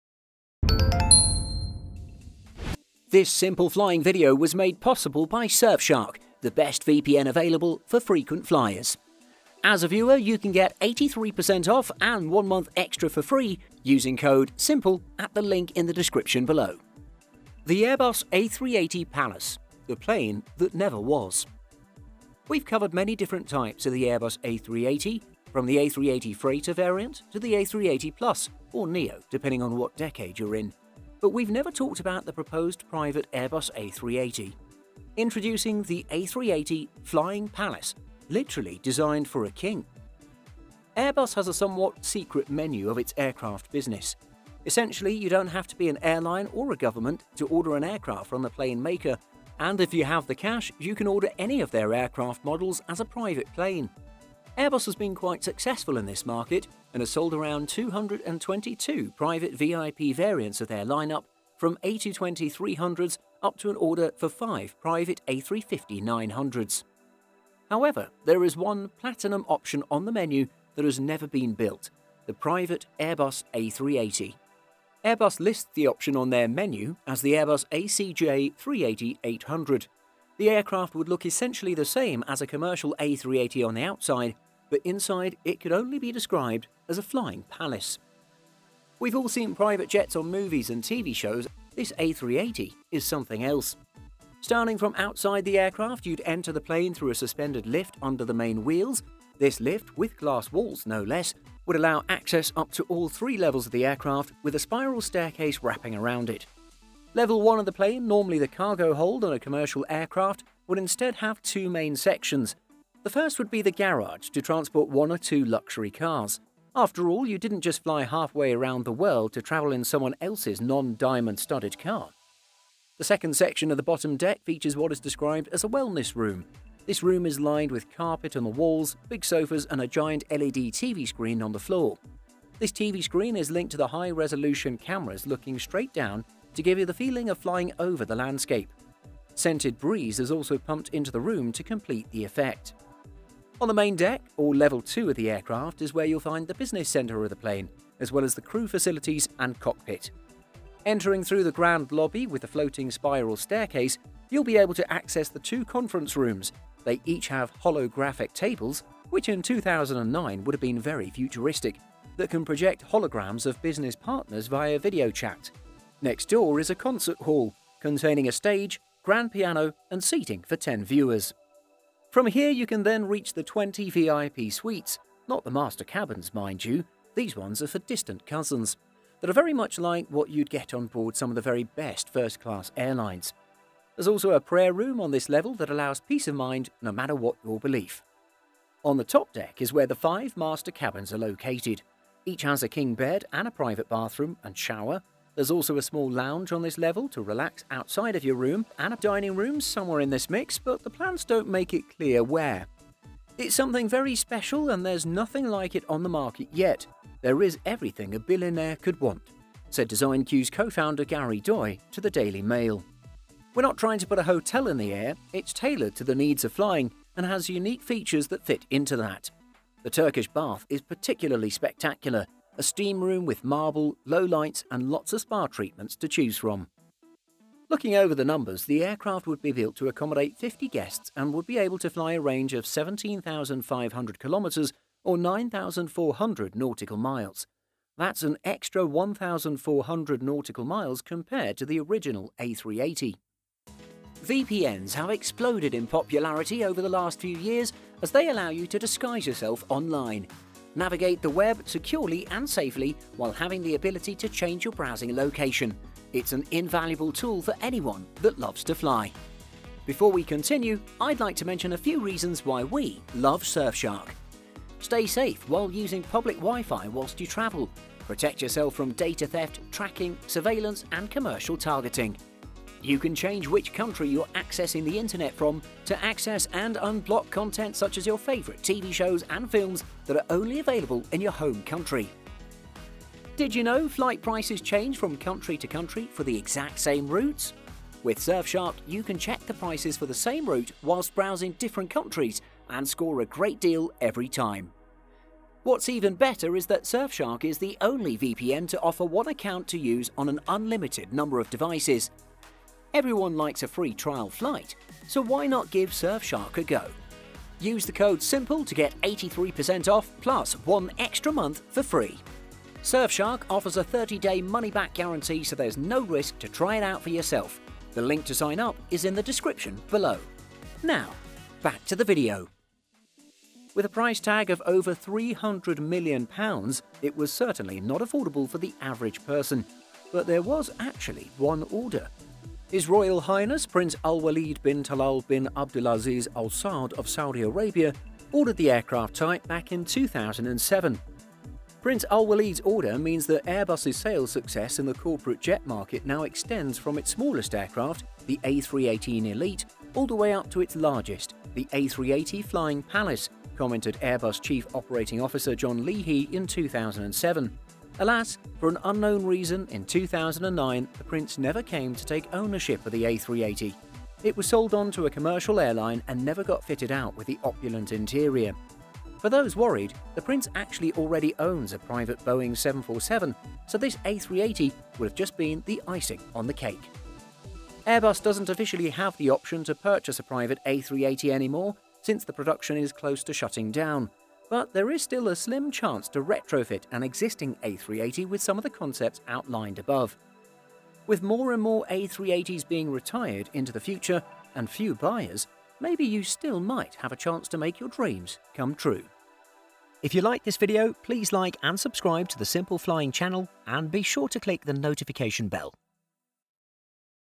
The Airbus A380 Palace: The Plane That Never Was
We have covered many different types of the Airbus A380, from the A380 freighter variant to the A380 plus (or neo, depending what decade you’re in), but we have never talked about the proposed private Airbus A380. Yes, you read that right, an entire A380 just for one person and their guests. Introducing, the A380 ‘flying palace’, literally designed for a king.